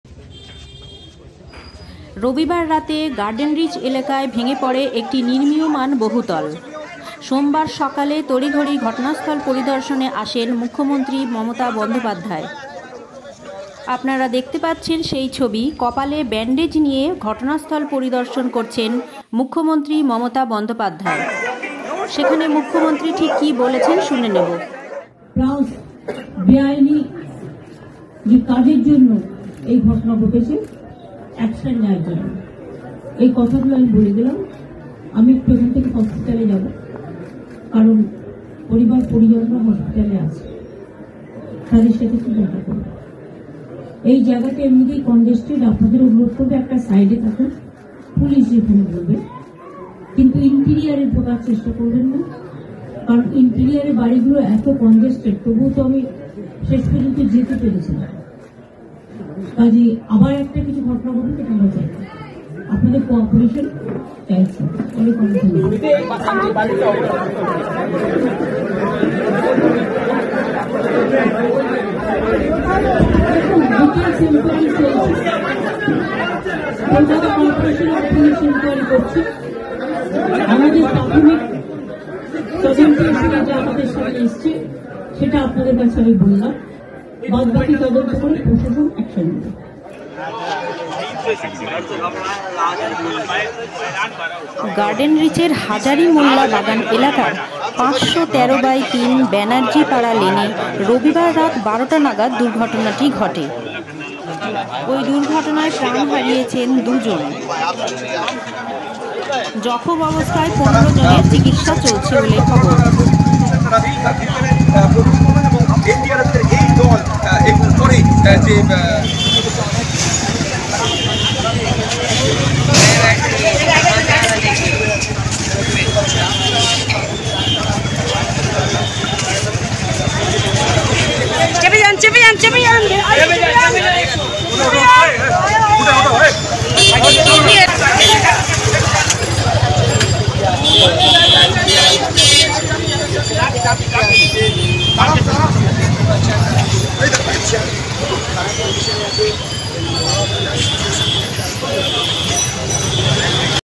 CM at Garden Reach| কপালে ব্যান্ডেজ নিয়ে গার্ডেনরিচে দুর্ঘটনাস্থল পরিদর্শনে মুখ্যমন্ত্রী
কপালে ব্যান্ডেজ নিয়ে গার্ডেনরিচে দুর্ঘটনাস্থল পরিদর্শনে মুখ্যমন্ত্রী